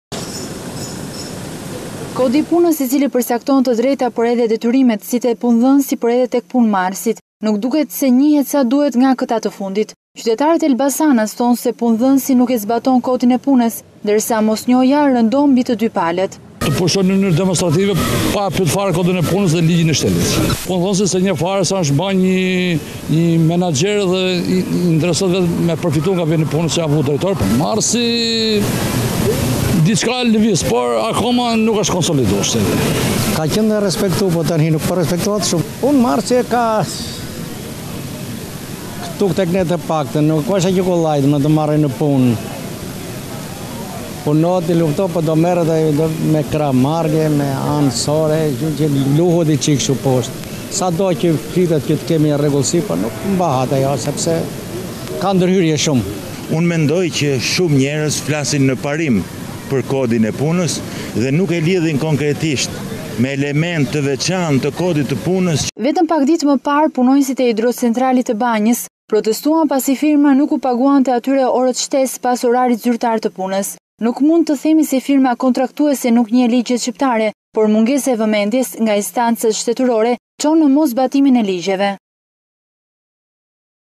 Elbasan , punëmarrësit nuk njohin Kodin e Punës- RTV Ora News- Lajmi i fundit-
Elbasan - Kodi i Punës i cili përcakton të drejtat dhe detyrimet si të punëdhënësit edhe të punëmarrësit, duket se nuk njihet nga të dy palët. Ndonëse në administratën shtetërore ai njihet dhe zbatohet pjesërisht,  ndërsa për punonjësit e punësuar në privat, as njihet e as zbatohet.

Qytetarët elbasanas thonë se punëdhënësi nuk e zbaton Kodin e Punës, por edhe në rastet kur punëmarrësi ka njohuri, ai nuk kërkon zbatimin e këtij kodi, pasi vendi i tij i punës rrezikohet. Qytetaret shtojnë se mosnjohja e ligjeve rëndon mbi të dy palët.